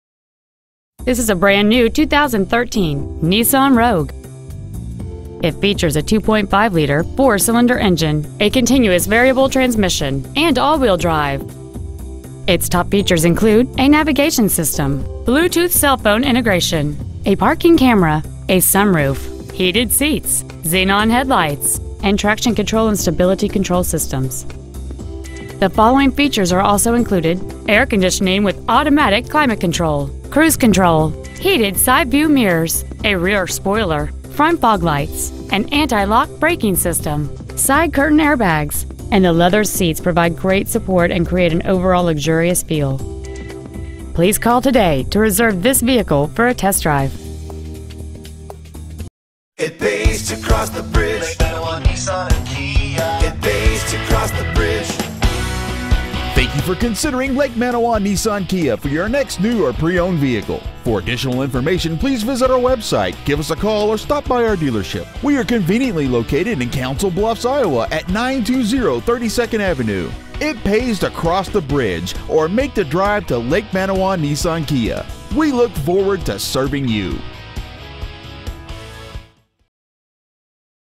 2013 Nissan Rogue Council Bluffs IA
Year : 2013
 Make : Nissan
 Model : Rogue
 Engine : 2.5L 4 cyls
 Trans . : Continuously Variable (CVT)
 Exterior : Cayenne Red
 Miles : 1

 Stock : T130909